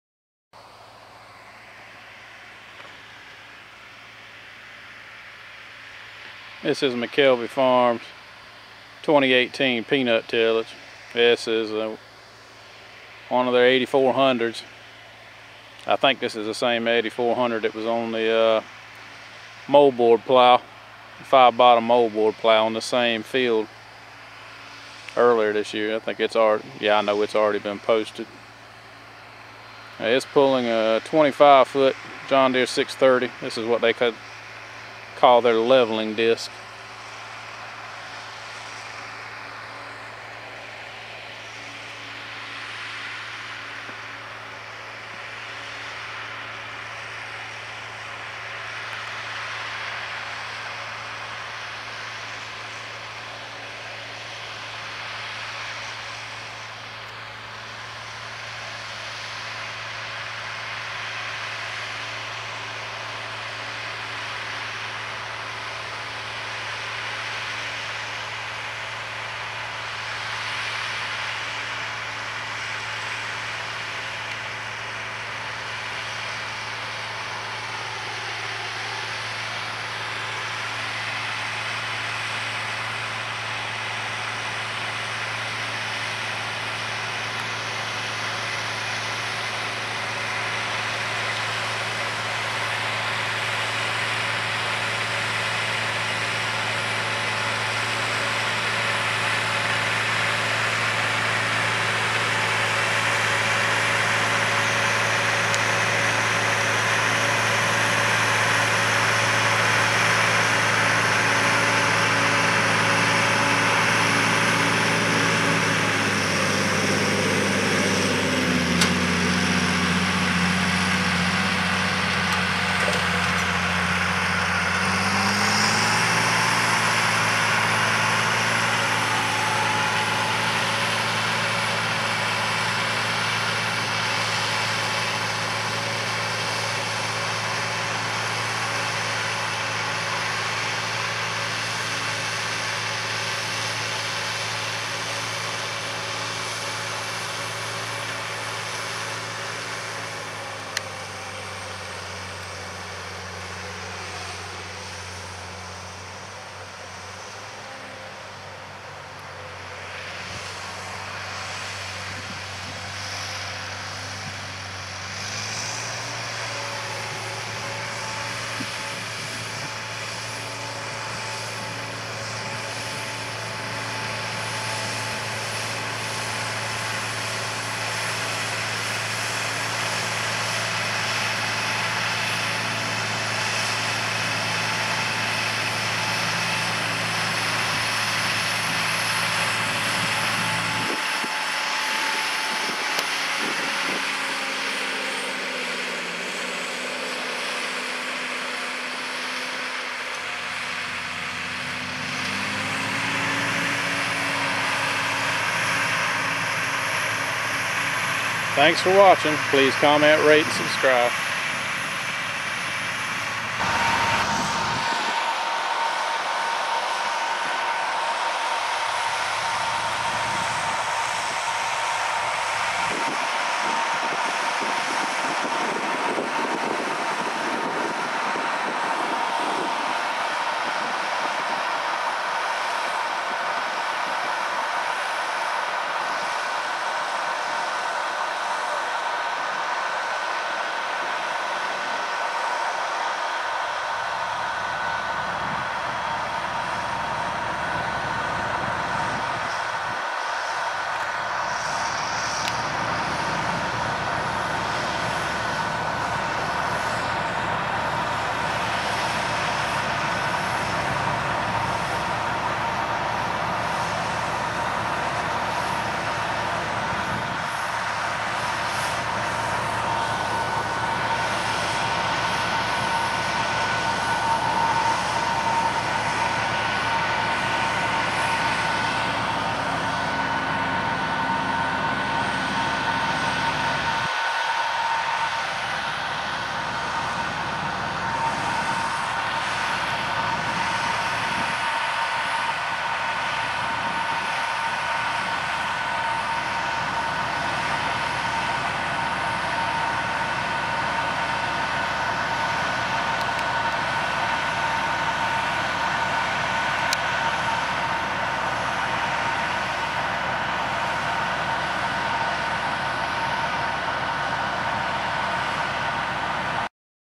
JOHN DEERE 8400 DISKING. McELVY FARMS 2018 DISKING
In this video McElvy Farms is seen disking some moldboard plowed ground just in front of their peanut planter. They are using their John Deere 840 with a 25 foot leveling disk to smooth out the field and kill some grass that has come up in the field.